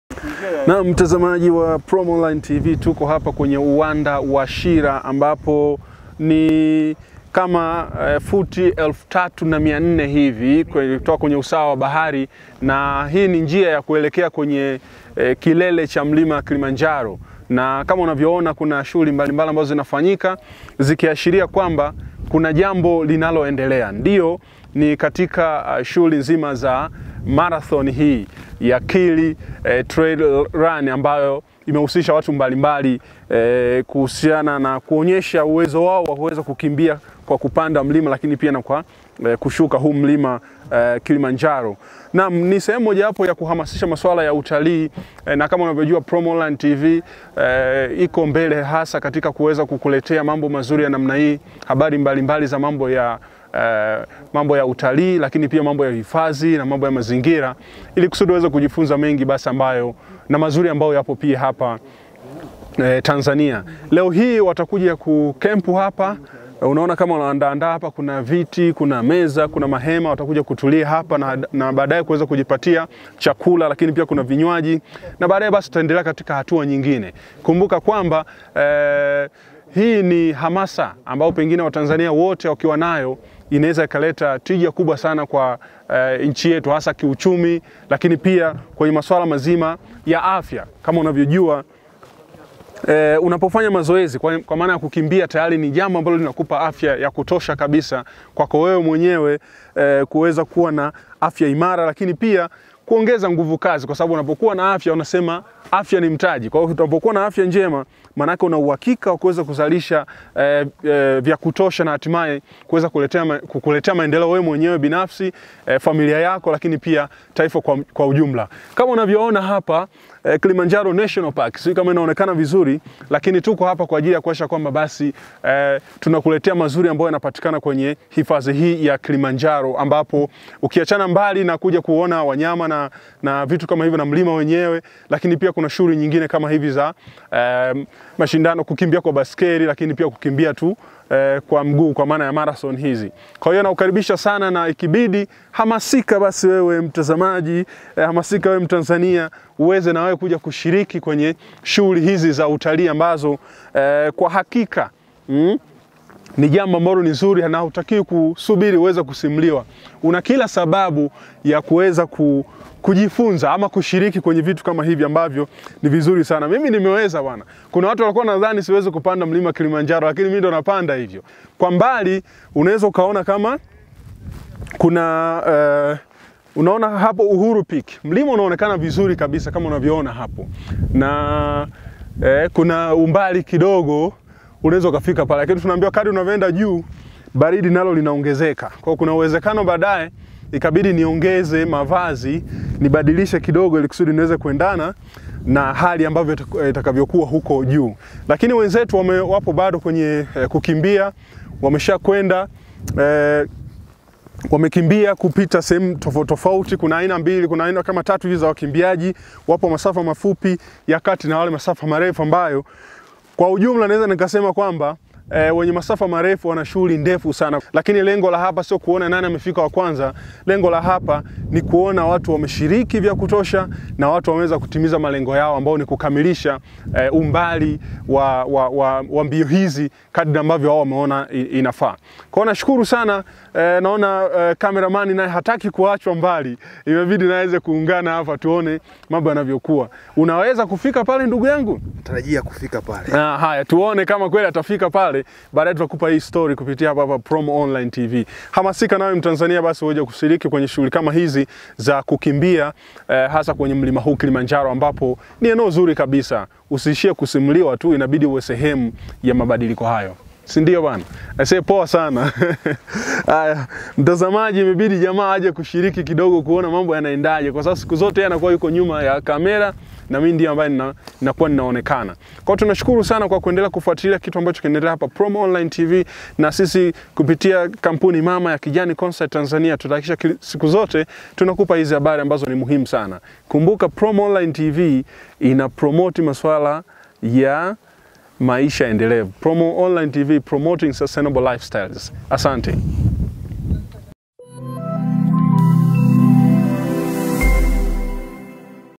Fuatilia simulizi tamu kutoka kwenye kambi ya wakimbiaji wa Kili Trail Run kwenye Mlima Kilimanjaro
@promoonlinetv7442 inakuletea habari, dondoo na makala za kilimobiashara, mazingira, maliasili na utalii